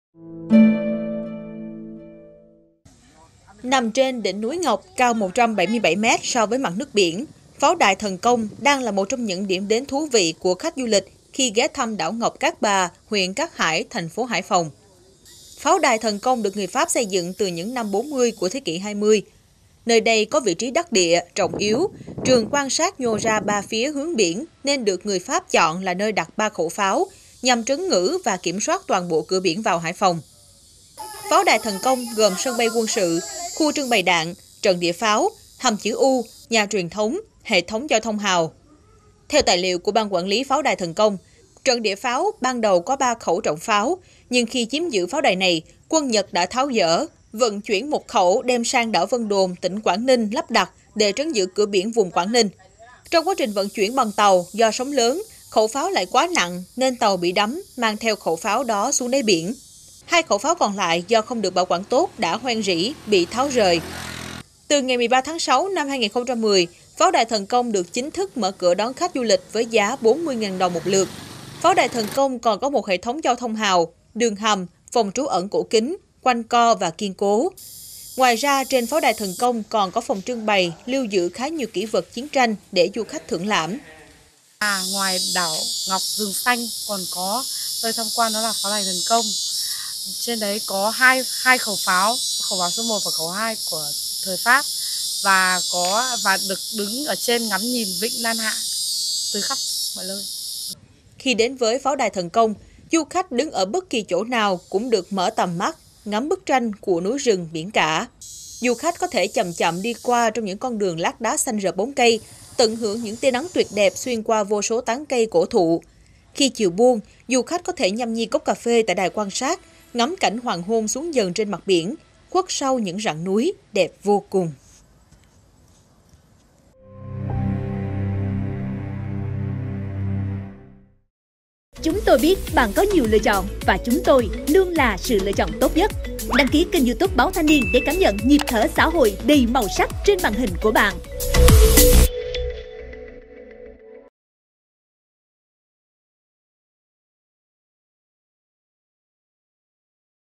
Ngắm đảo ngọc Cát Bà tuyệt đẹp từ Pháo đài Thần Công
Nằm trên đỉnh núi Ngọc cao 177m so với mặt nước biển, Pháo đài Thần Công đang là một trong những điểm đến thú vị của khách du lịch khi ghé thăm đảo ngọc Cát Bà, H. Cát Hải, TP.Hải Phòng.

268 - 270 Nguyễn Đình Chiểu, Phường 6, Q.3, TP HCM